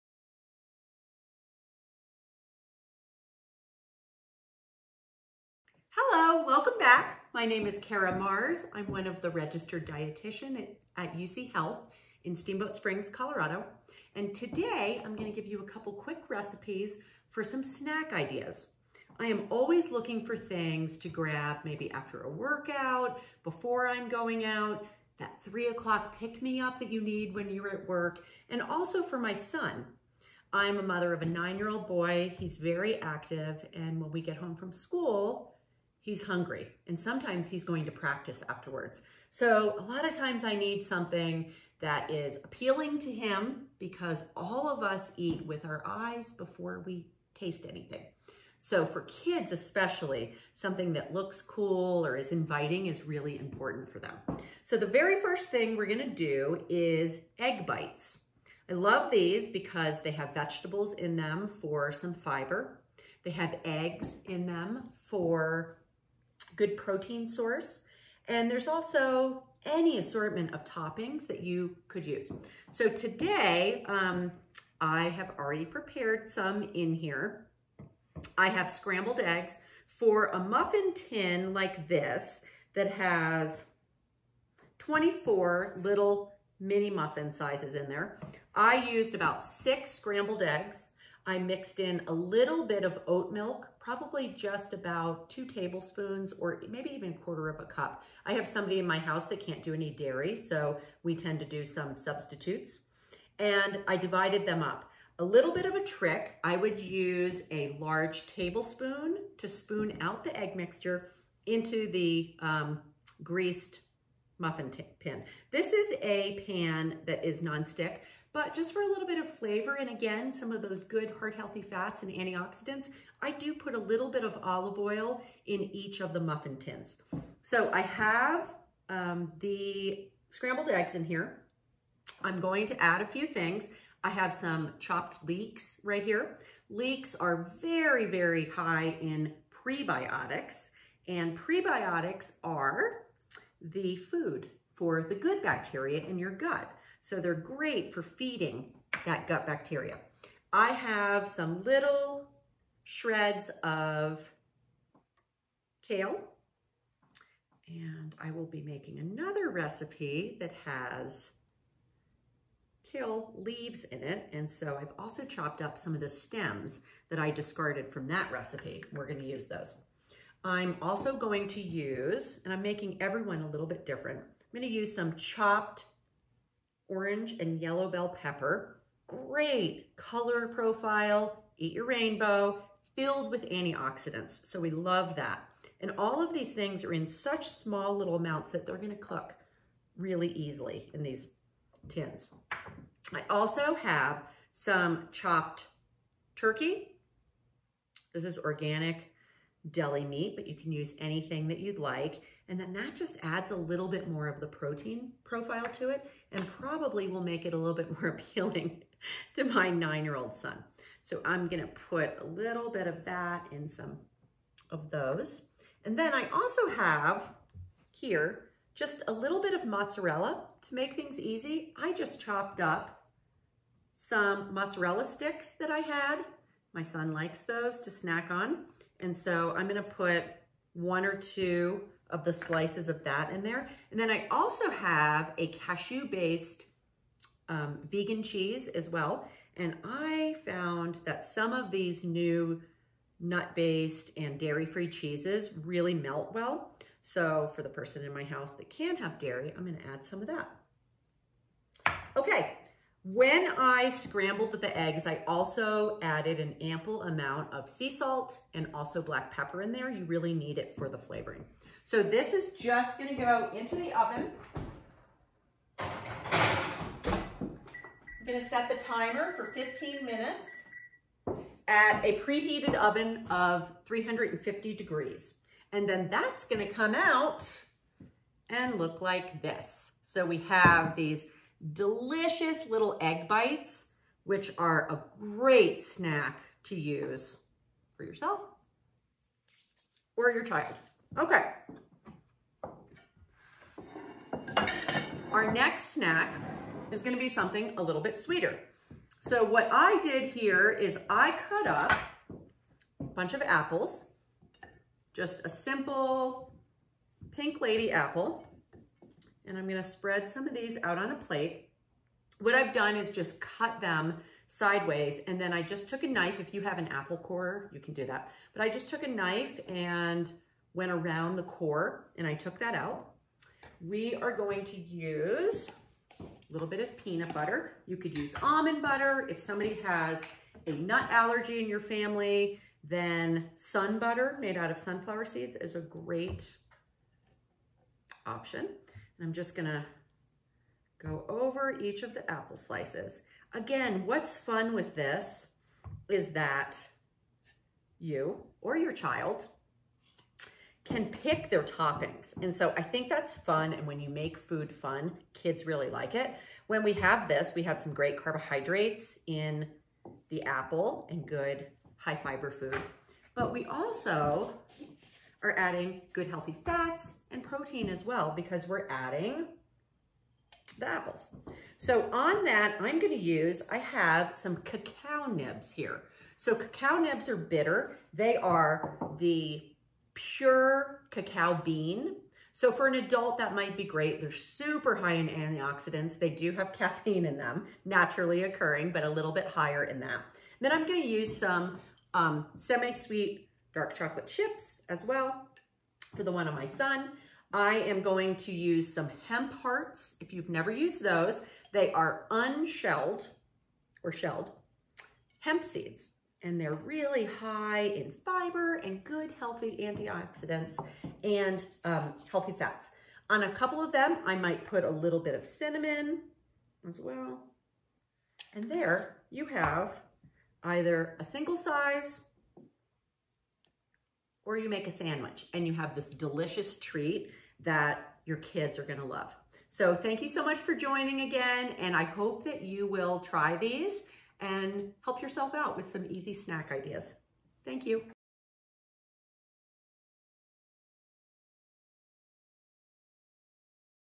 Eat the Rainbow with Kid-Friendly Snacks | evre | UCHealth
Kids use their eyes to choose what they want to eat. Cara Marrs, a Registered Dietician with UCHealth, teaches us to make some healthy snacks that look great.